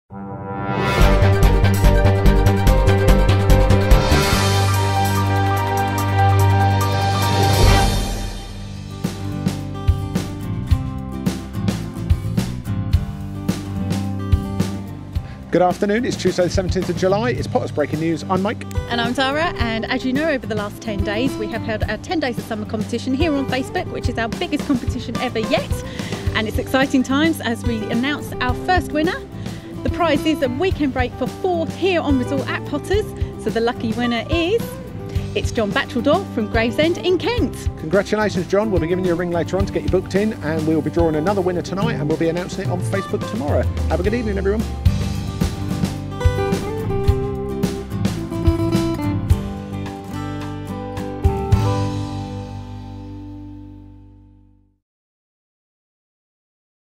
Potters 10 Days of Summer Competition 1st Winner Announced.
Mike and Tara announce the first lucky winner of a family weekend break to Potters Leisure Resort, from our 10 Days of Summer Competition...